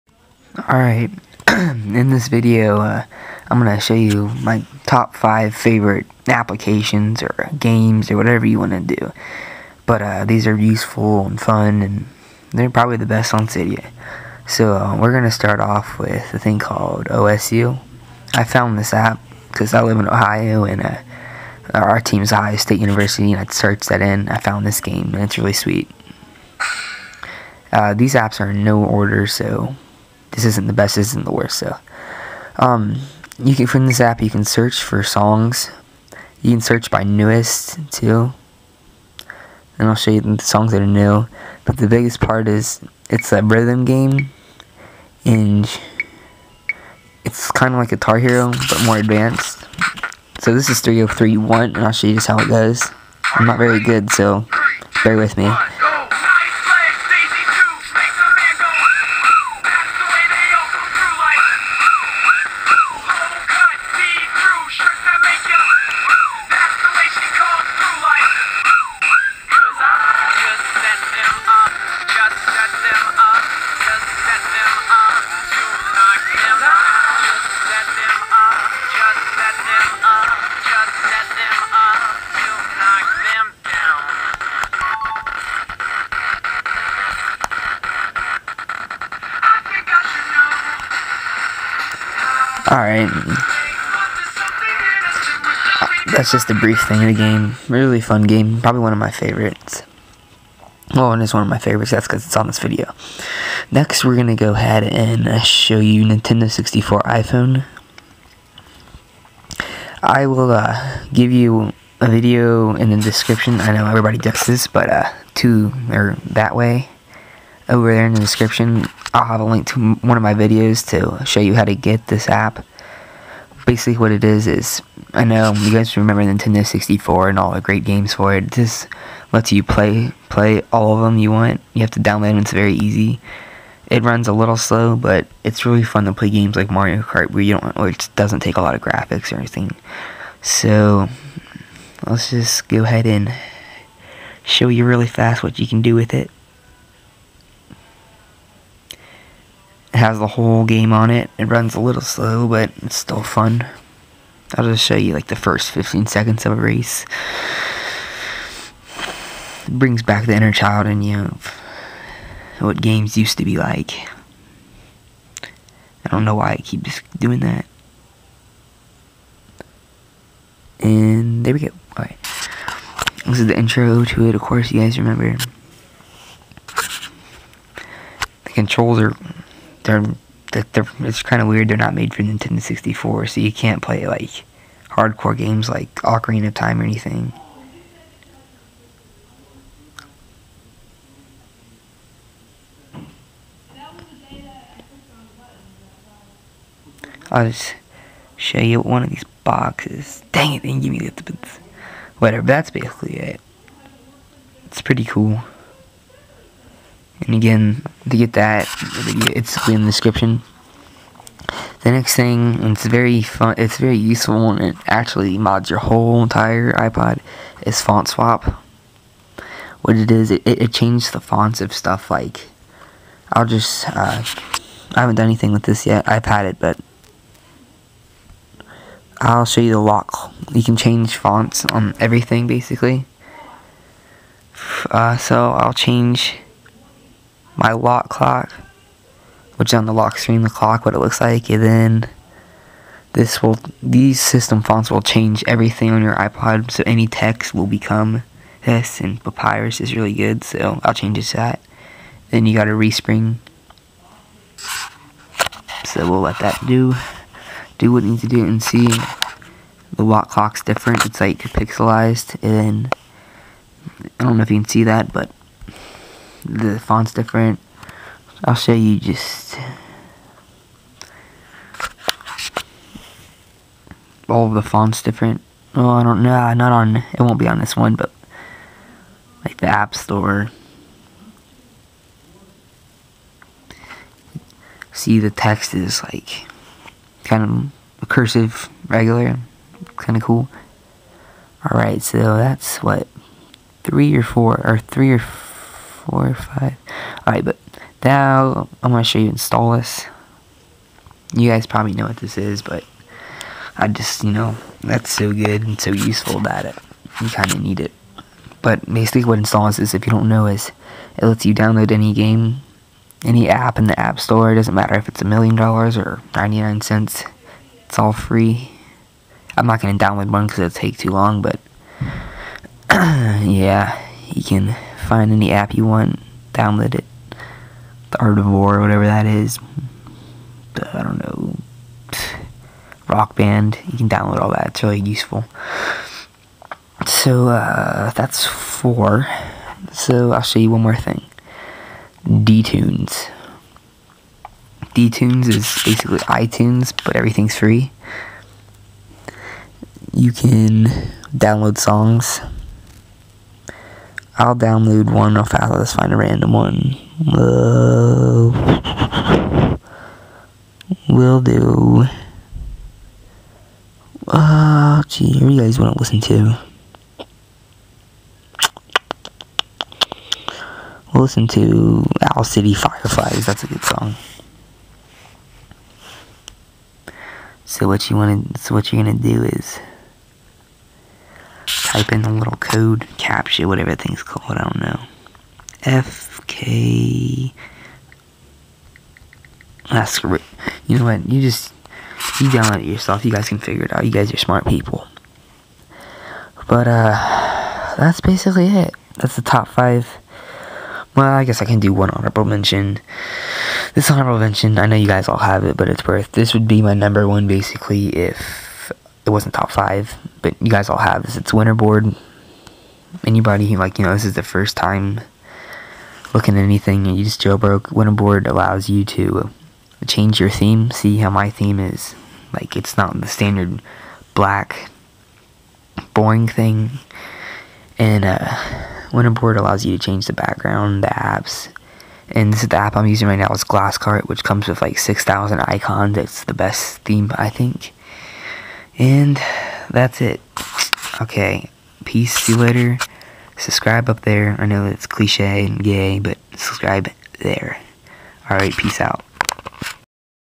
Top 5 Best Cydia Apps for 2010
These are the apps that are the most fun and most useful for cydia.

Tops 5 Cydia Apps

1.Osu!
3.FontSwap
4.Installous
5.dTunes

Honorable Mention: WinterBoard